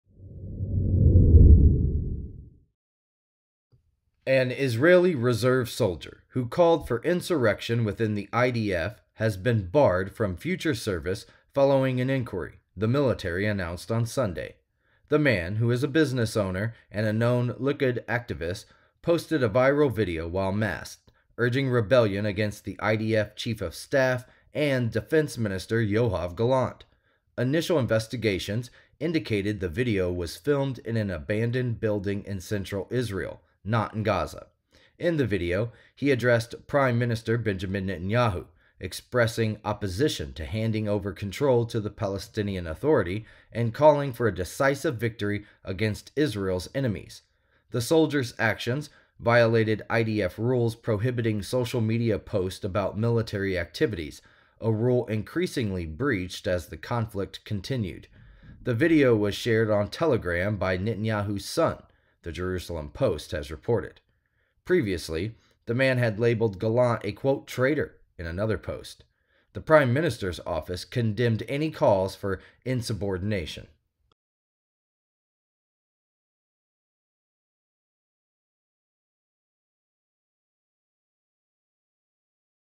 IDF probes masked soldier’s threatening video calling for mutiny
A reserve soldier who called for insurrection within the IDF has been barred from future service following an inquiry, the military announced on Sunday.

The man, a business owner from Herzliya and a known Likud activist, posted a viral video while masked, urging rebellion against IDF Chief of Staff Herzi Halevi and Defense Minister Yoav Gallant.

Initial investigations indicated the video was filmed in an abandoned building in central Israel, not Gaza.

In the video, he addressed Prime Minister Benjamin Netanyahu, expressing opposition to handing over control to the Palestinian Authority and calling for a decisive victory against Israel’s enemies.

The soldier’s actions violated IDF rules prohibiting social media posts about military activities, a rule increasingly breached as the conflict continued.

The video was shared on Telegram by Netanyahu’s son, Yair Netanyahu, the Jerusalem Post has reported.